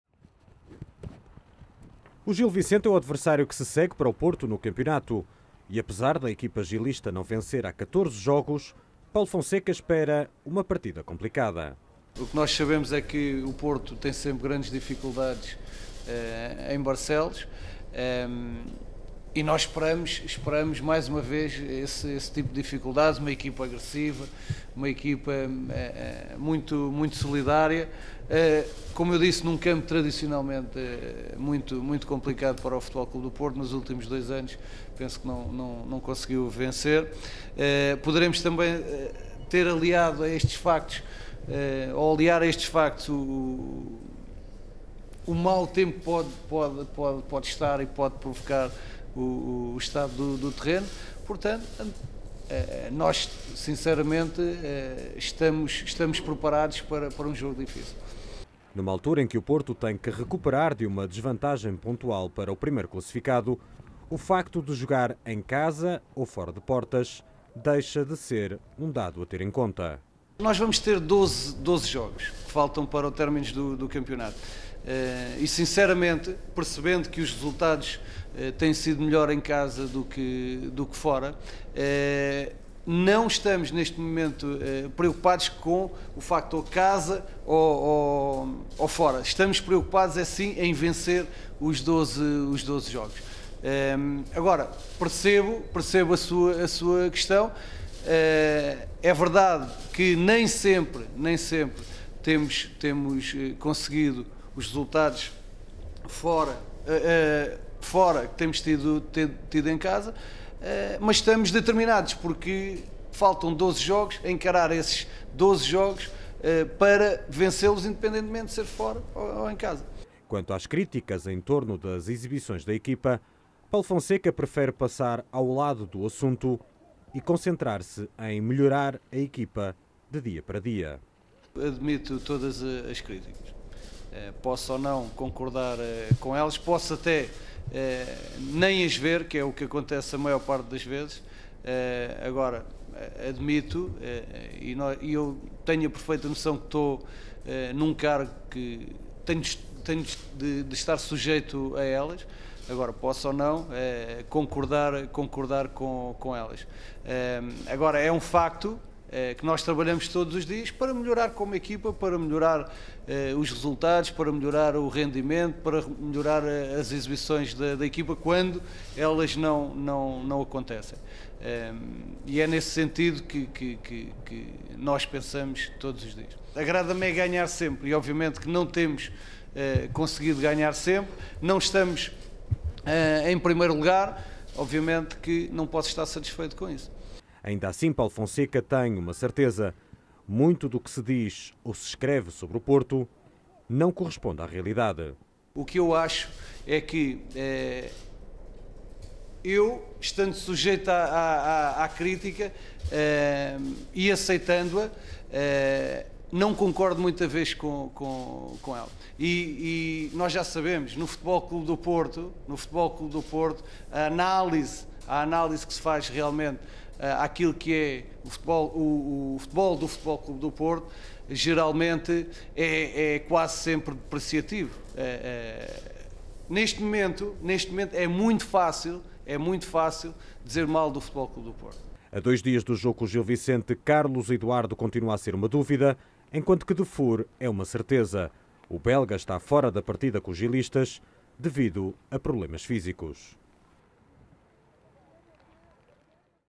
Futebol: Paulo Fonseca (conferência de imprensa do Gil Vicente-FC Porto, 19.ª jornada da Liga)
​O técnico do FC Porto não se fia nos 14 jogos sem vencer do Gil Vicente e sustenta-se na história recente para antever dificuldades na deslocação a Barcelos (domingo, 19h15), da 19.ª jornada da Liga. Paulo Fonseca garante uma equipa focada no seu trabalho e determinada em aliar bons resultados a boas exibições.